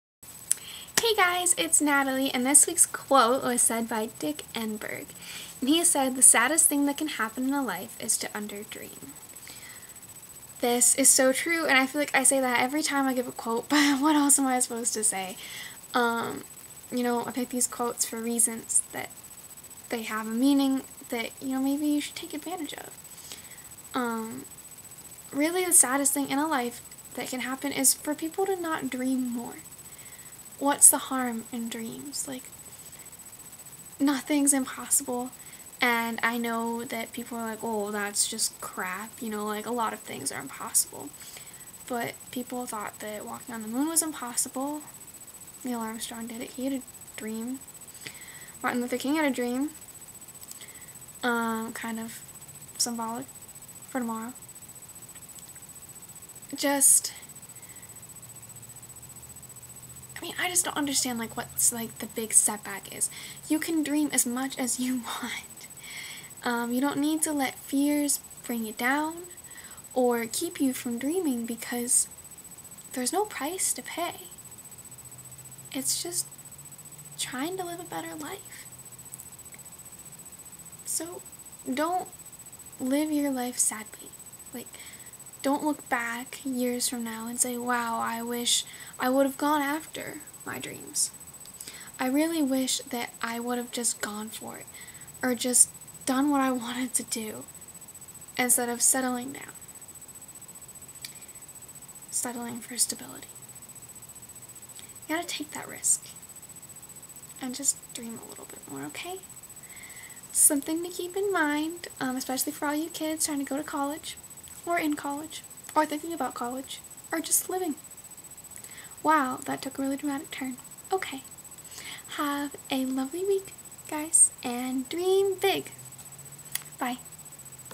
Empowering Tip #52: Dream more
This week’s quote is from Dick Enberg, a famous sportcaster: “The saddest thing that can happen in a life is to under dream.” As it seems I always say, this is so true – but then, I pick these quotes for reasons; they have meaning, and they are worth taking advantage of.

Many people won’t dream big, they feel it’s not worth it – but what’s the harm in dreams? Nothing’s impossible. People may think that a lot of things are impossible; but really, are they? We’ve walked on the moon; and symbolic of tomorrow, Martin Luther King had a dream – a big one, and it came true.

Don’t let fears bring you down or keep you from doing something. Don’t live your life sadly; don’t look back years from now and wish you would have gone after your dreams. Take that risk now – dream a little bit more.